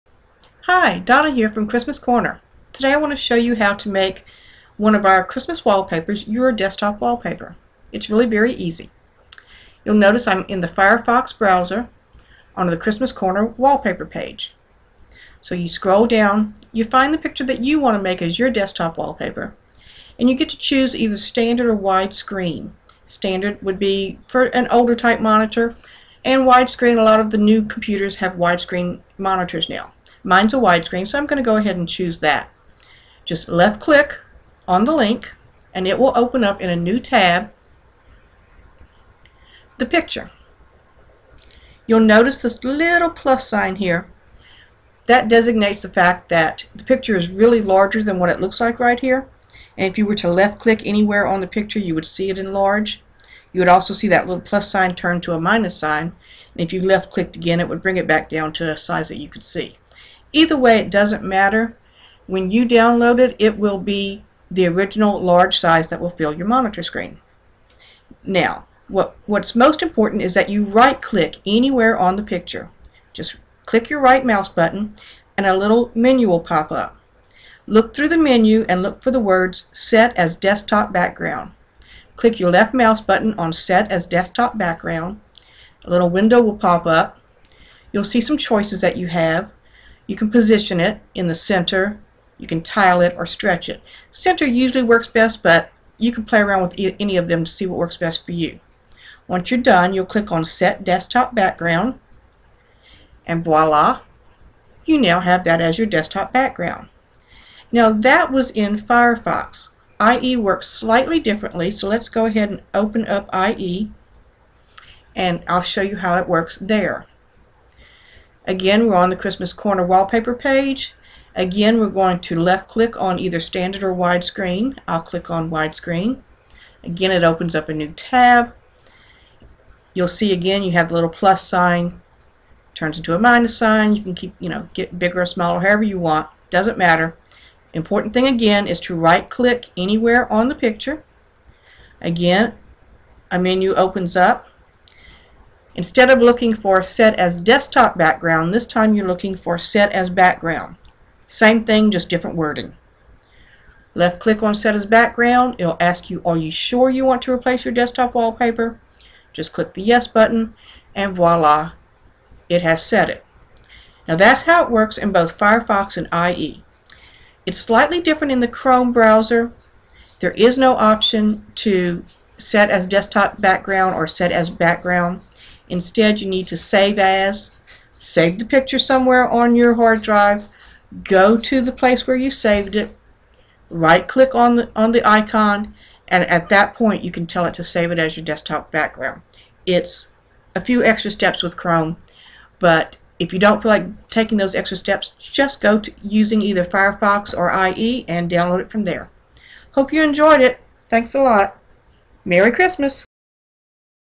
How To Save Christmas Wallpaper as Your Desktop Background
: Watch how easy it is to save one of Christmas Corner's Christmas wallpapers as your computer's desktop background.